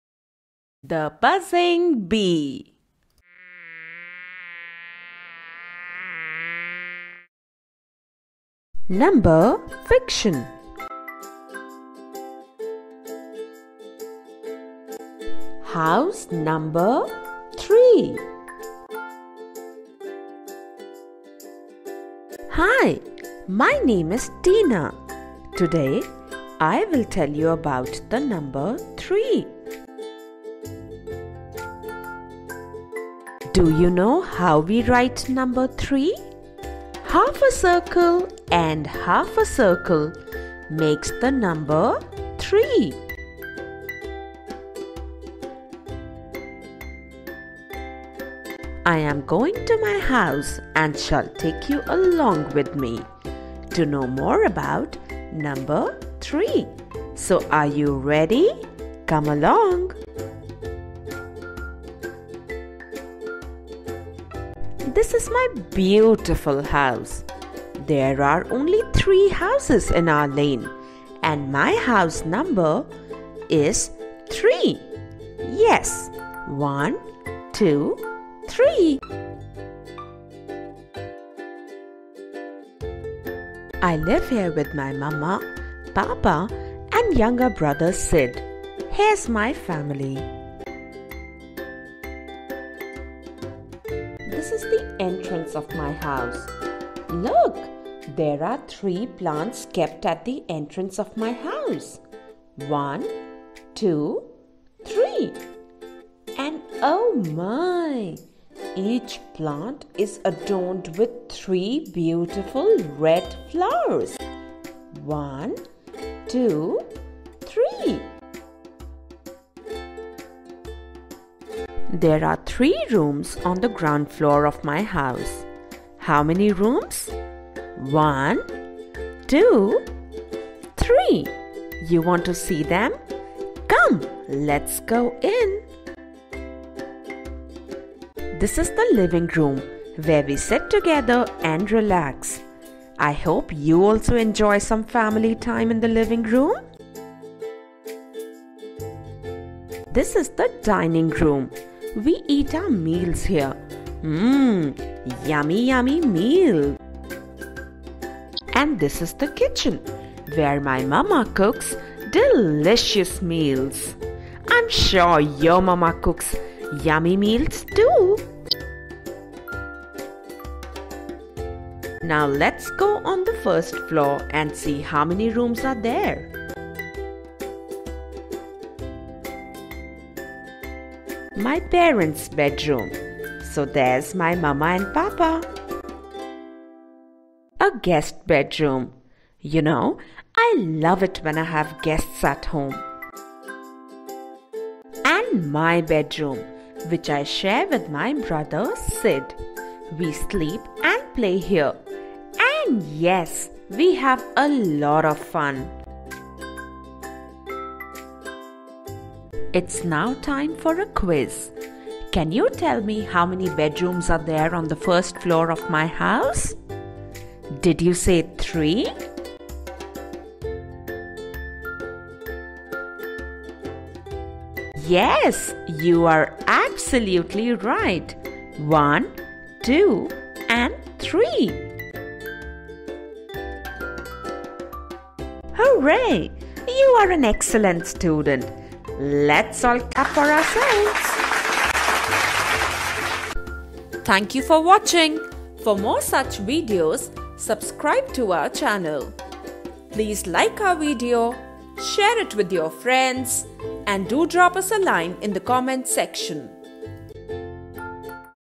Learn to Count Numbers through Stories | Number 3 Story for Kindergarten Kids
Number Stories are a fun way for kindergarten kids to learn to count numbers. Kids enjoy learning number concepts through stories and further enhance their vocabulary. The Buzzing Bee YouTube Channel is a great educational tool for kids, parents and teachers.